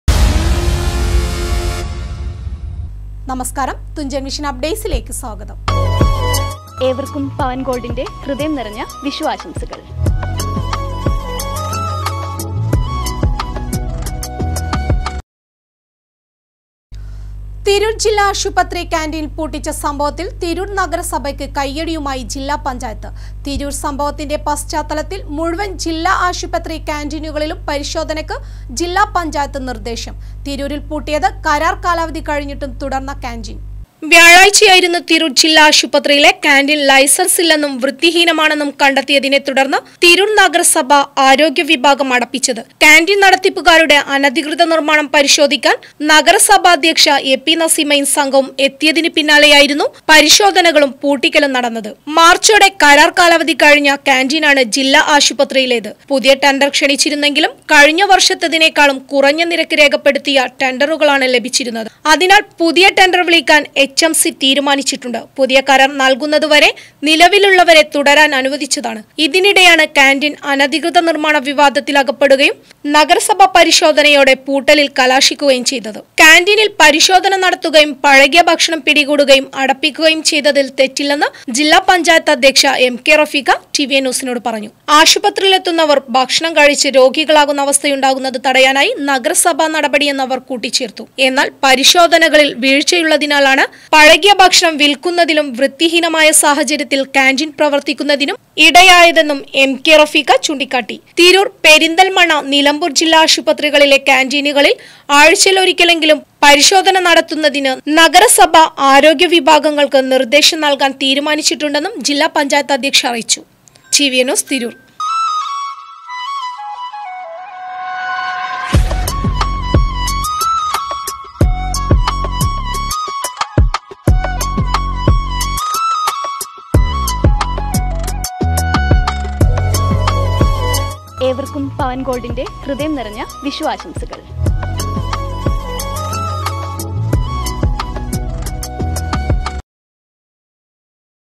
കാന്റീന്‍ പൂട്ടിച്ച സംഭവത്തില്‍ തിരൂര്‍ നഗരസഭക്ക് കയ്യടിയുമായി ജില്ലാ പഞ്ചായത്ത്
തിരൂര്‍ ജില്ലാ ആശുപത്രി കാന്റീന്‍ പൂട്ടിച്ച സംഭവത്തില്‍ തിരൂര്‍ നഗരസഭക്ക് കയ്യടിയുമായി ജില്ലാ പഞ്ചായത്ത്. തിരൂര്‍ സംഭവത്തിന്റെ പശ്ചാത്തലത്തില്‍ മുഴുവന്‍ ജില്ലാ ആശുപത്രി കാന്റീനുകളിലും പരിശോധനക്ക് ജില്ലാ പഞ്ചായത്ത് നിര്‍ദേശം. തിരൂരില്‍ പൂട്ടിയത് കരാര്‍ കാലാവധി കഴിഞ്ഞിട്ടും തുടര്‍ന്ന കാന്റീന്‍.